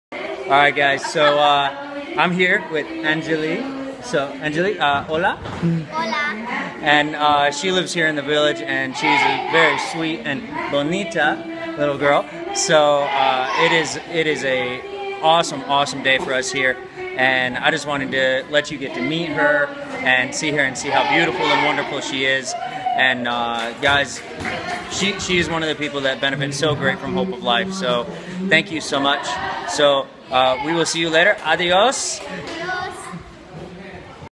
Roads Less Traveled goes to Guatemala - Part 6
Meet Angelina, a wonderful and loving little girl who is full of life. She is just one of over 300 kids in the village, almost 2/3 of whom still need a sponsor. Hope of Life International - check it out!